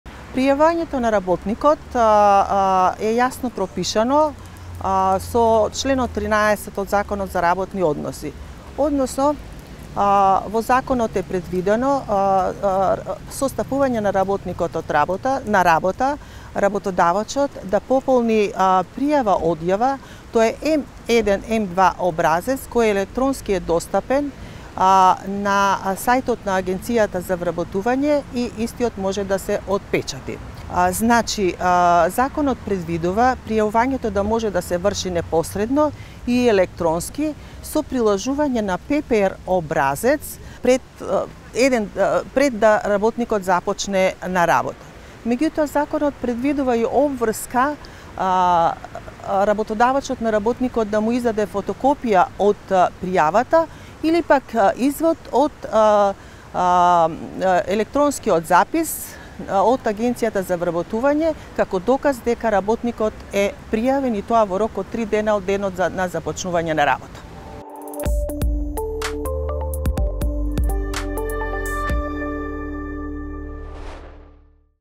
Пријавување на вработен на пола работно време: може ли да се направи електронски или мора на шалтер?
Одговара Мирјанка Алексовска, експерт за креирање политики од областа на трудовото право.